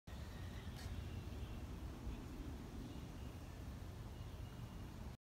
Push Up Plus
Start in a push up position with the arms extended. Push your hands into the floor causing your shoulder blades to slide towards your sides. Release back down pinching your shoulder blades to your spine. Repeat.